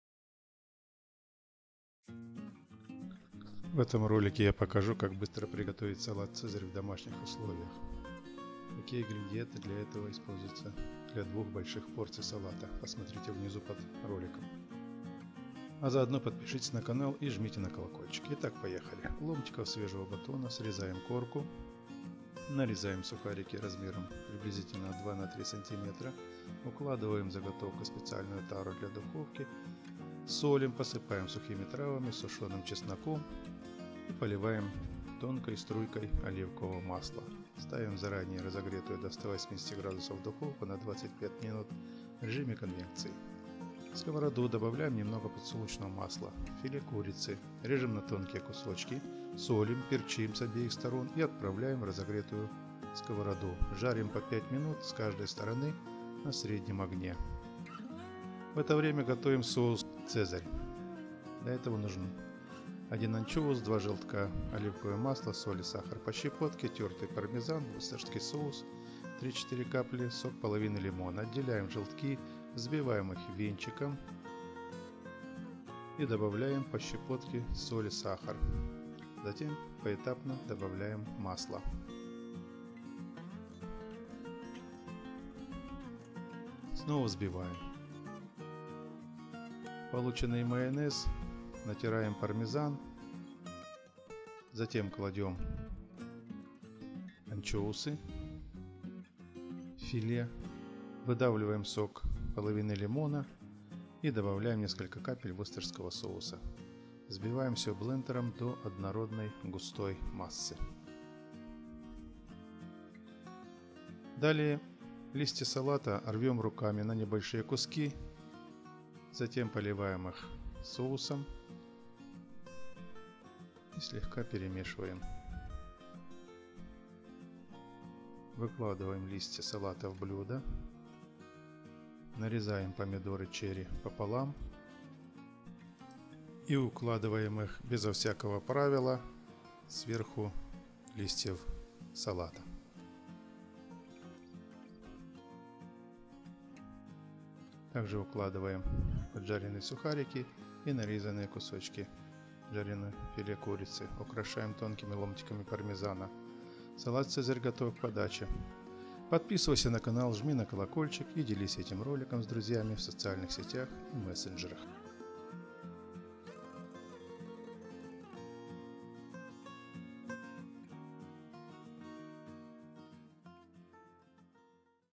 Салат Цезарь, как приготовить в домашних условиях
Салат Цезарь, как приготовить в домашних условиях. Подробный рецепт приготовления известного салата Цезарь.
Ингредиенты:
Для двух порций салата Цезарь нужно взять приблизительно:
листья салата – 300 г
специально приготовленные сухари – 100-120 г
филе курицы – 250 г
масло для жарки – 2 ст. ложки
соль и перец для филе – по вкусу
пармезан для украшения – 20 г;
– для соуса:
желтки свежих яиц – 2 шт.
масло оливковое – 150 мл
соль – ¼ ч. ложки
сахар – щепотка
пармезан – 50 г
анчоус без костей – 1-2 шт.
вустерский соус – ¼ ч. ложки
сок лимона – 1 ст. ложка.
Я прошу прощения, если автоматический переводчик не совсем корректно перевел описание и название видео на ваш язык.